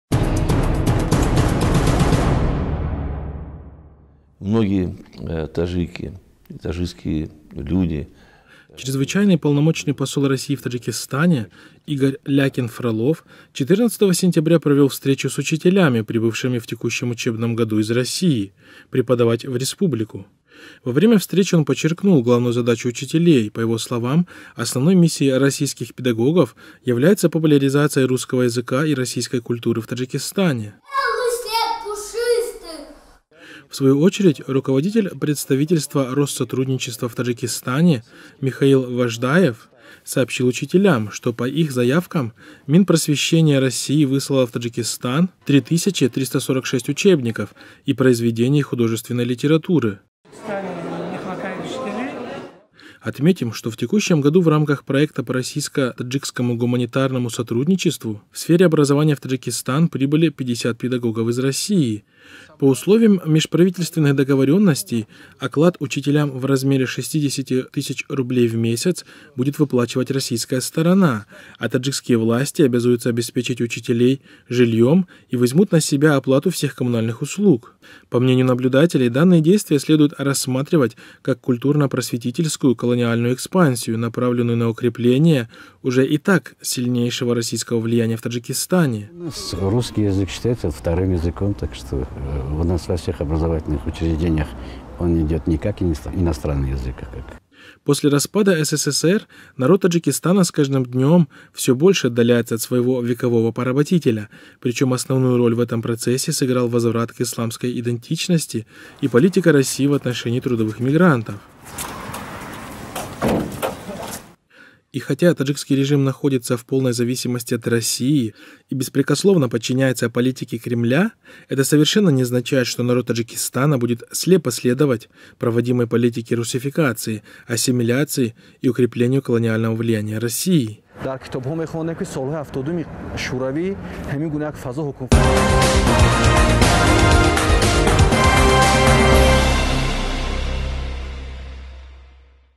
Таджикистан: популяризация русской культуры и языка
Чрезвычайный и Полномочный посол России в Таджикистане Игорь Лякин-Фролов 14 сентября провел встречу с учителями, прибывшими в текущем учебном году из России преподавать в республику. Во время встречи он подчеркнул главную задачу учителей: по его словам, основной миссией российских педагогов является популяризация русского языка и российской культуры в Таджикистане.